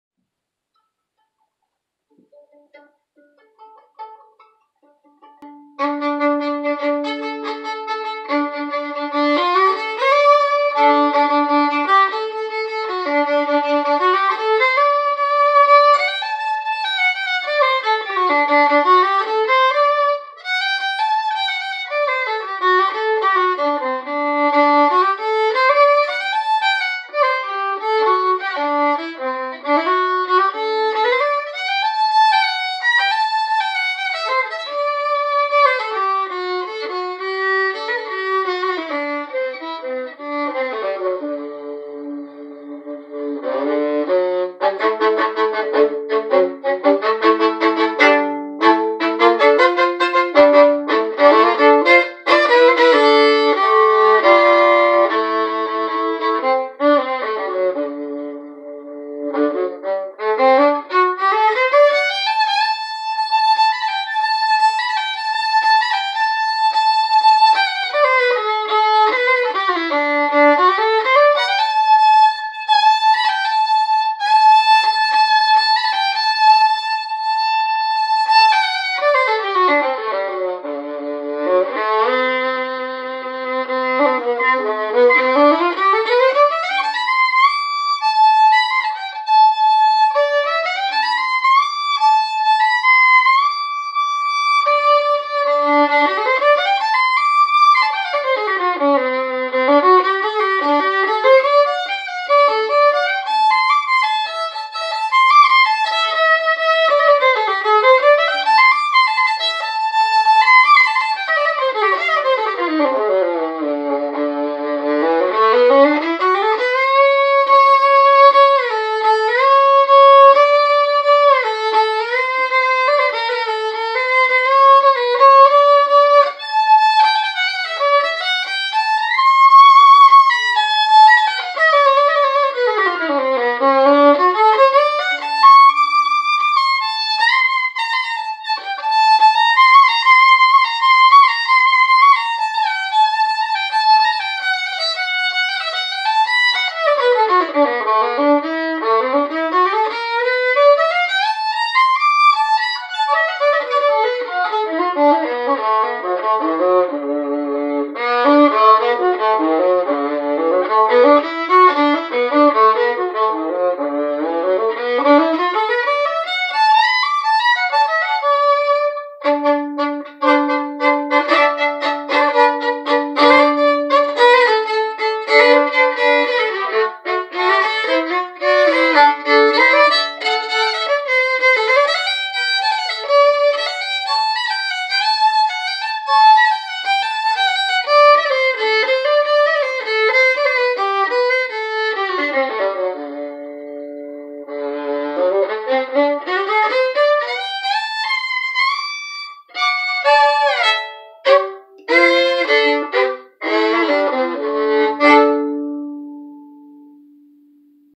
Yinfente 5-string acoustic/electric violin - improv themes for practice
I got this Yinfente 5-string violin 2 years ago and have been using it as my main instrument for live and studio work since that time. It has metal guitar-like tuners which made it possible to switch the tailpiece for a 'harp' style slanted one. This gave a little extra tension to hold the low 5th string in tune, tho it is still a bit wobbly. The instrument is also much louder with the tuner-less tailpiece.

This was recorded directly with a phone without further mixing, to give a raw representation of the acoustic sound. The pickup is built into a slot for the bridge. To record with the pickup or to use it with an amp, it does not require batteries and it uses a standard 1/4" jack (visible on the side near the shoulder rest).

I compared it to a few other affordable violins (both 4 and 5 strings, electric/silent and electric/acoustic) and stuck with this one for the overall reliability and sound.

(This is an improv performance, inspired by jigs, reels, ragas, and etudes)
(I have been learning violin/fiddle since early 2018)
(This is my first youtube upload)